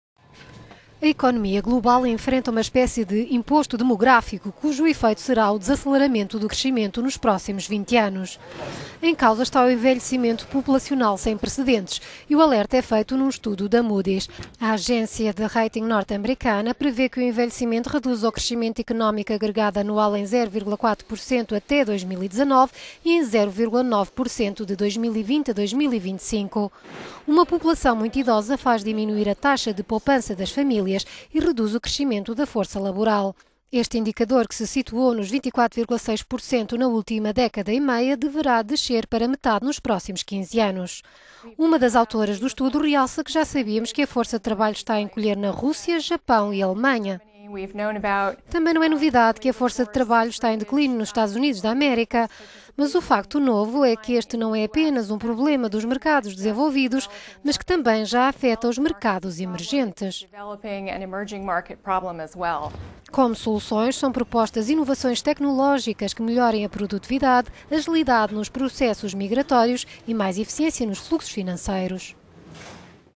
Economia mundial enfrenta "imposto" demográfico - economy
A economia global enfrenta uma espécie de "imposto" demográfico, cujo efeito será o desaceleramento do crescimento  nos próximos 20 anos. Em causa está o envelhecimento populacional sem precedentes e o alerta é feito num estudo da Moody's.

A agência de rating norte-americana  prevê que o envelhecimento populacional reduza o crescimento económico agregado anual em 0,4% até 2019, e em 0,9% de 2020 a 2025.

Uma população muito idosa faz diminuir a taxa de poupança das famílias e reduz o crescimen…